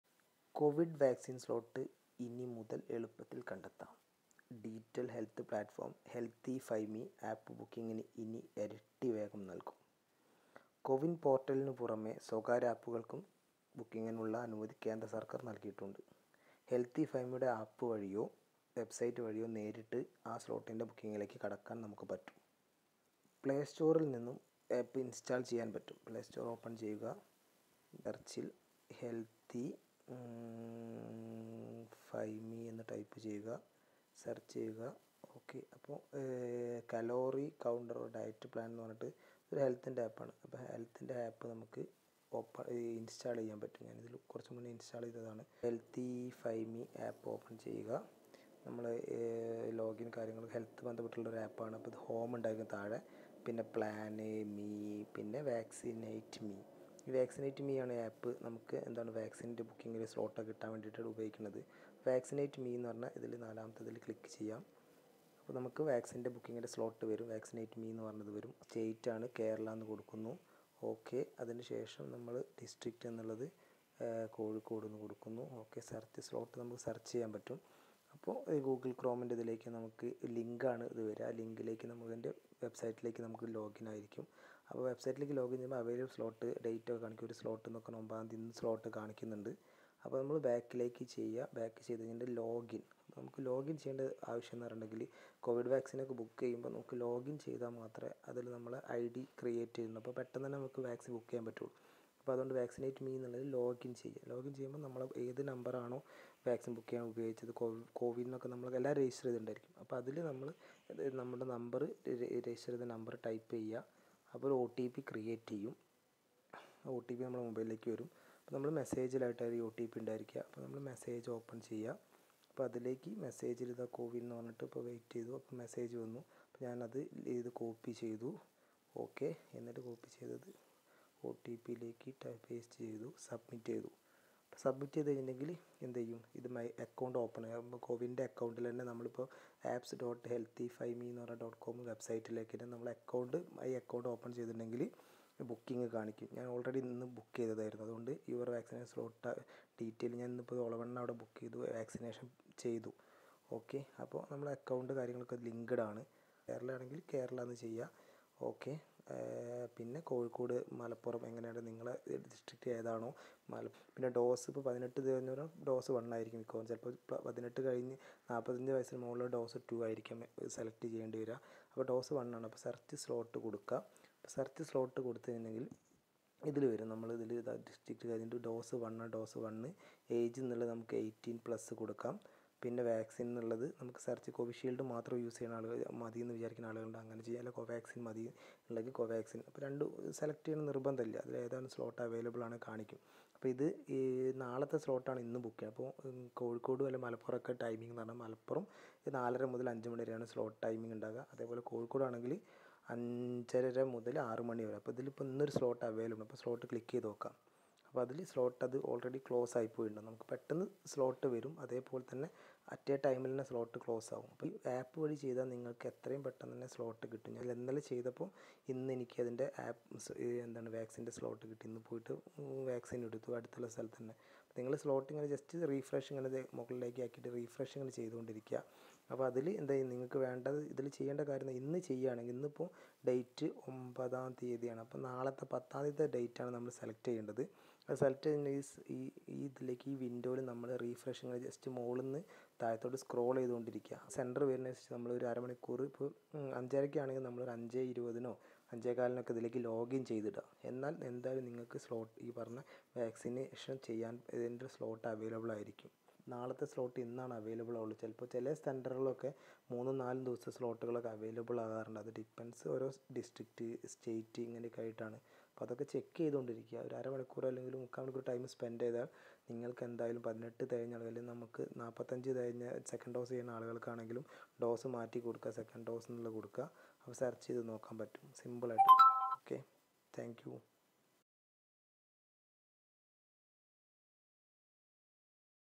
ഇനി കോവിഡ് വാക്സീൻ കിട്ടിയില്ല എന്ന് പറയരുത് | How to get a COVID-19 VACCINE | HEALTHIFYME | KERALA
Download app link : HealthiFyMe

COVID-19 vaccines are here, but when will you and your loved ones be able to get the shot? And how do you find an appointment?